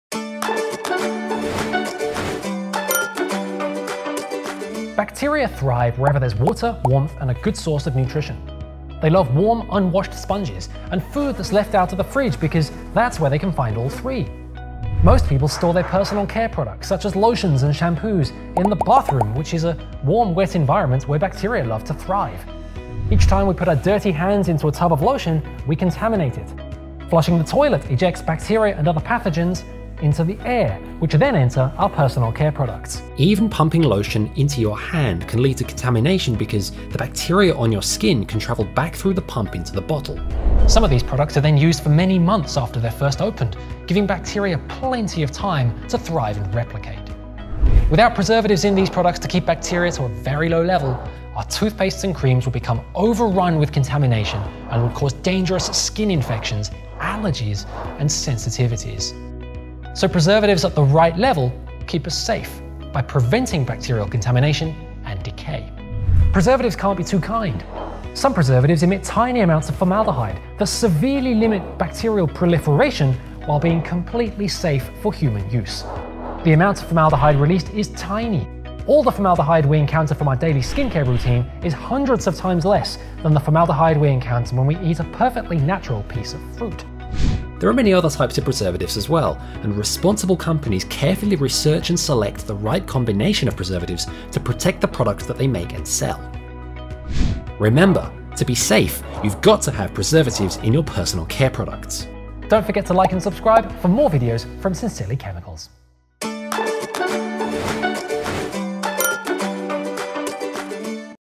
Why do we need Preservatives in our Personal Care Products?
Bacteria thrive wherever there is water, warmth and a good source of nutrition. They love warm, unwashed sponges and food that’s left out of the fridge because that’s where they can find all three: water, warmth and a good source of nutrition.

Most people store their personal care products such as lotions and shampoos in the bathroom, which is a warm, wet environment where bacteria love to thrive. Each time we put our dirty hands into a tub of lotion, we contaminate it. Flushing the toilet ejects bacteria and other pathogens into the air, which then enter our personal care products.  Even pumping lotion onto your hand can lead to contamination because bacteria on your skin can travel back through the pump into the bottle.

Some of these products are used for many months after they’re first opened, giving bacteria plenty of time to thrive and replicate.
Without preservatives in these products to keep bacteria to a very low level, our toothpastes and face creams would become over-run with contamination and would cause dangerous skin infections, allergies and sensitivities. Preservatives at the right level keep us safe by preventing bacterial contamination and decay.

Preservatives can’t be too kind. Some preservatives that emit tiny amounts of formaldehyde and severely limit bacterial proliferation while being completely safe for human use. The amount of formaldehyde released is tiny – all the formaldehyde we encounter from our daily skincare routine is hundreds of times less than the natural formaldehyde we encounter when we eat a completely natural piece of fruit.  There are many other kinds of preservatives, and responsible companies carefully research and select the right combination of preservatives to protect the products that they make and sell.

Remember! To be safe, you’ve got to have preservatives in your personal care products!

REFERENCES

CONCLUSION

• Bacteria are everywhere, especially kitchens and bathrooms.
• Bacteria can be beneficial, harmless or harmful
• We must have preservatives in our personal care products